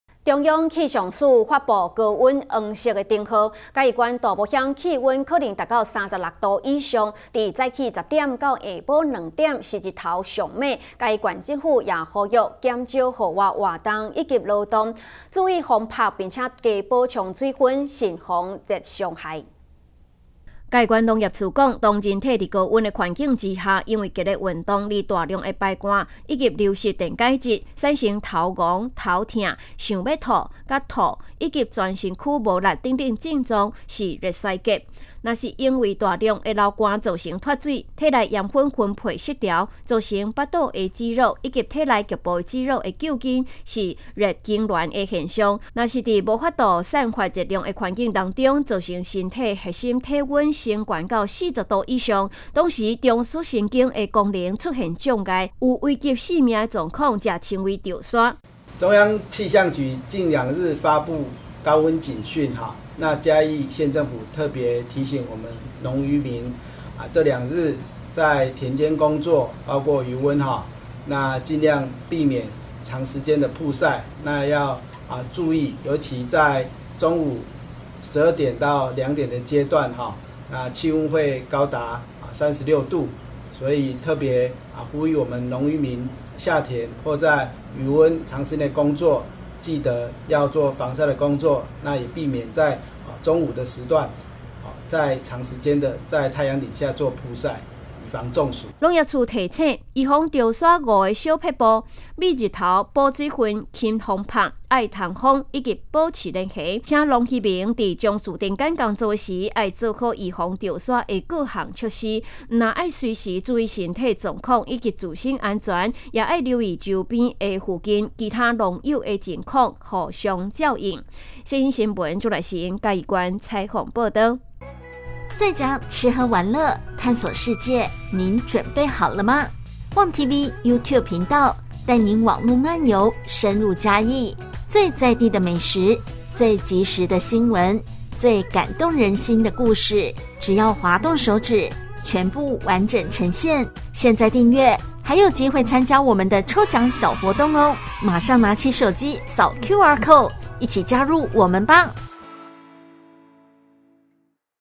世新新聞 氣候高溫炎熱 嘉縣府籲請農漁民工作預防中暑
113年3月26日 (二)  世新新聞

氣候高溫炎熱 嘉縣府籲請農漁民工作預防中暑

中央氣象署發布高溫黃色燈號，嘉義縣大埔鄉氣溫可能達36℃以上，於早上10時至下午2時太陽最烈，嘉義縣政府呼籲減少戶外活動及勞動，注意防曬並多補充水分，慎防熱傷害。